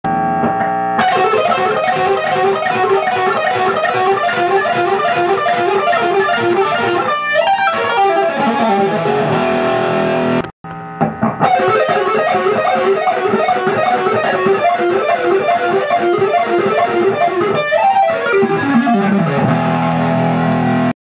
JEFF LOOMIS Diminished Lick
i was bored so i decided to upload this,i didnt play the full diminished lick,i just play the first part of it and end with a diminished tapping.